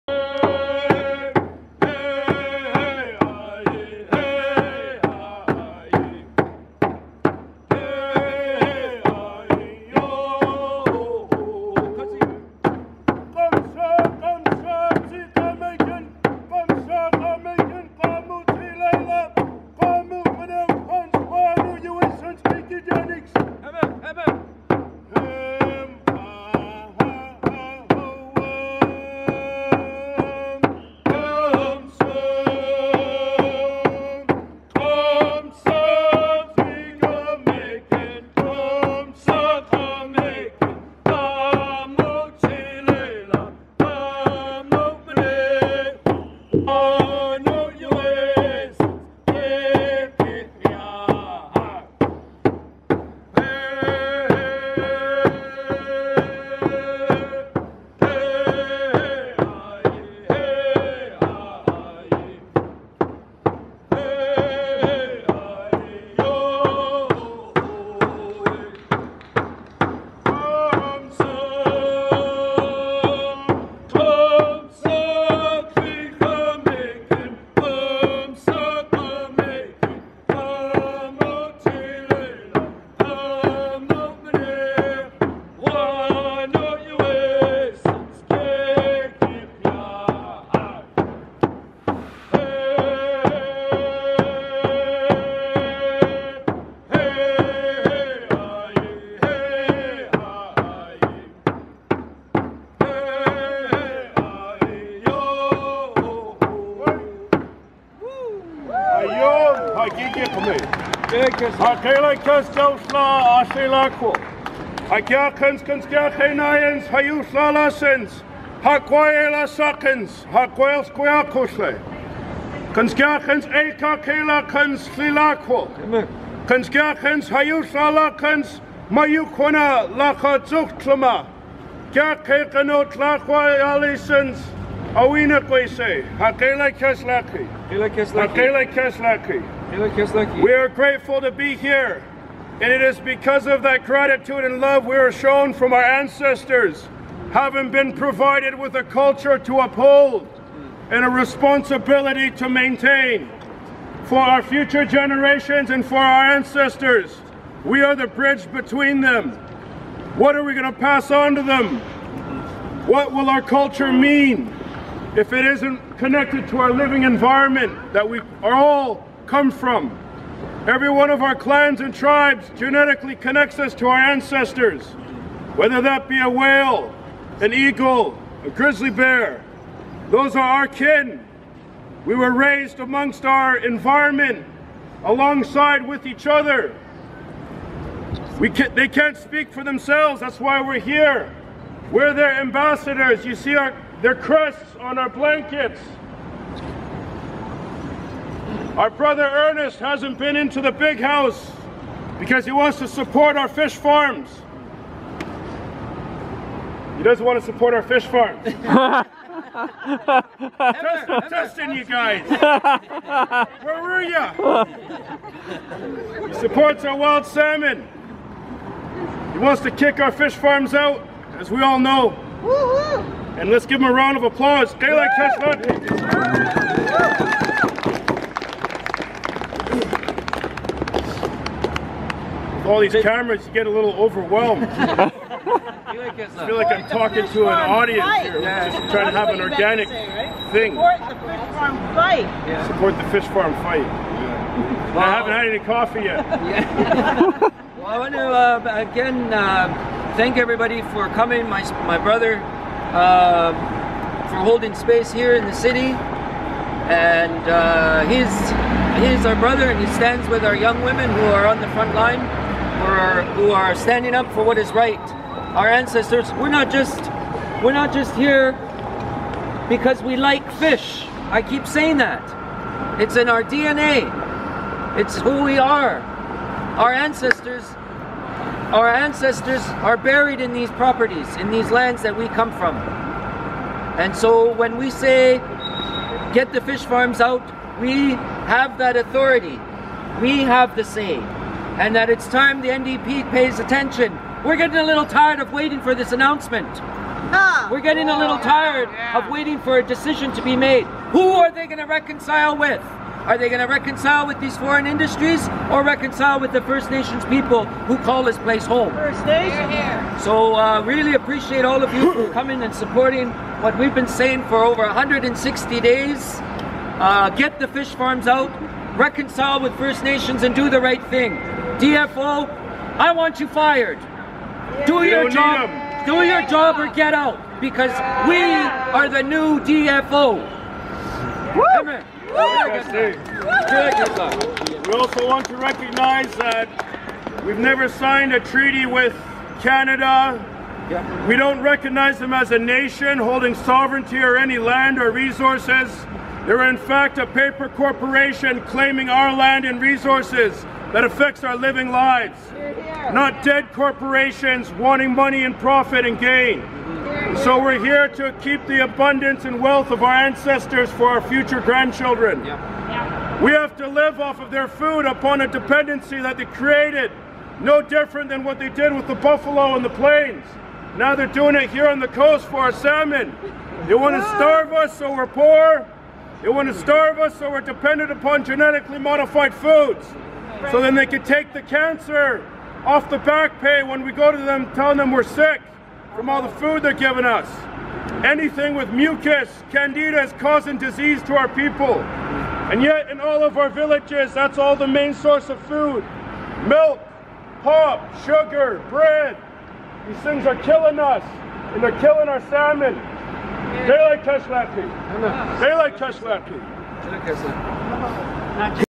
Wild Salmon Rally Supporting First Nations Fish Farm Fight
January 30, 2018 Canada Place Vancouver BC